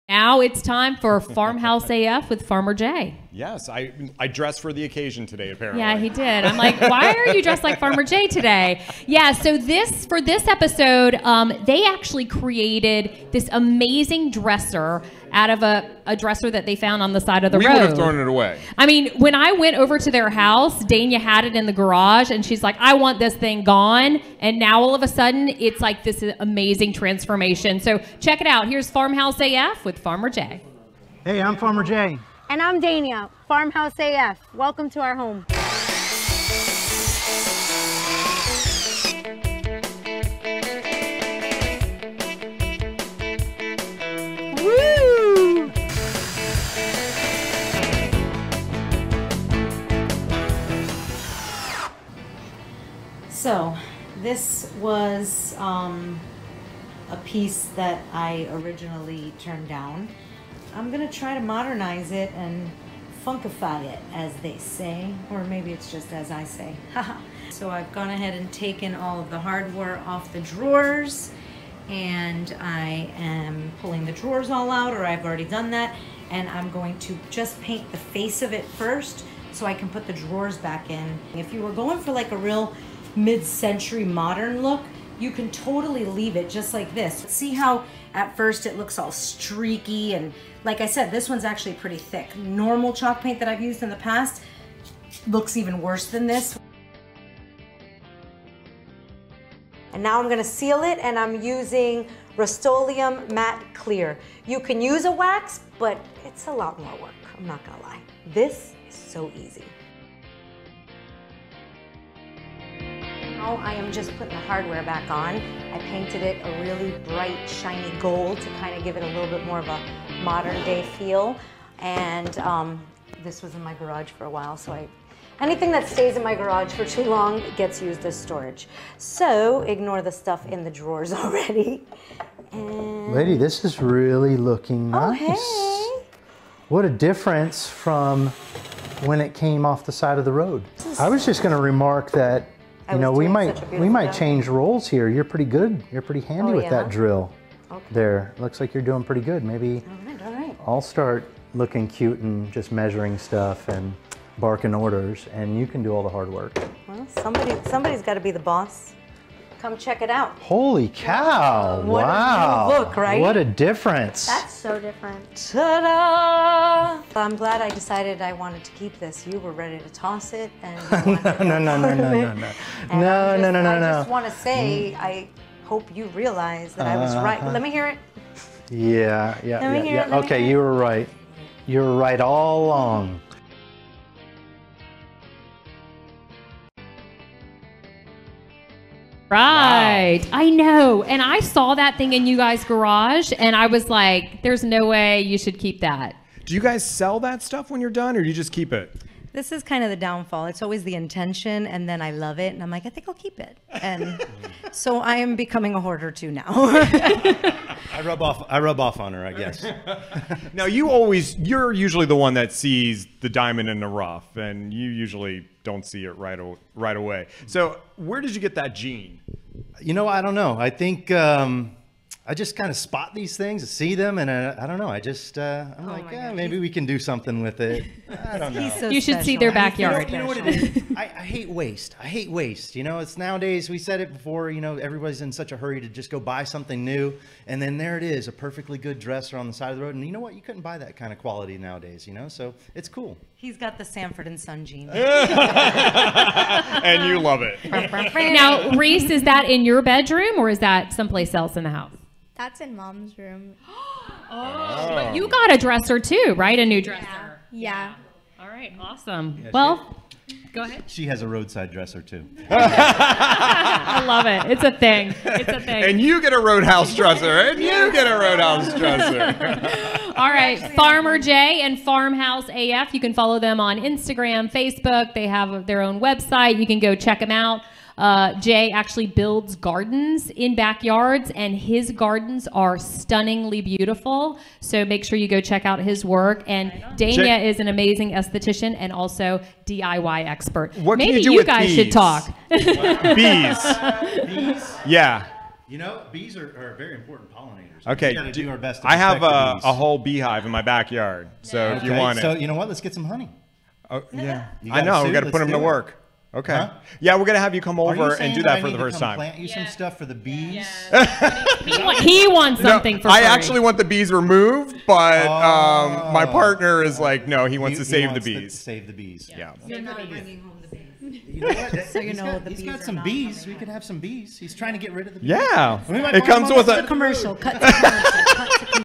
Farmhouse AF with Farmer Jay: Dresser Makeover
Watch how Farmhouse AF with Farmer Jay transform a dresser left for the dump into one of their favorite pieces in their home! Plus, they join us LIVE!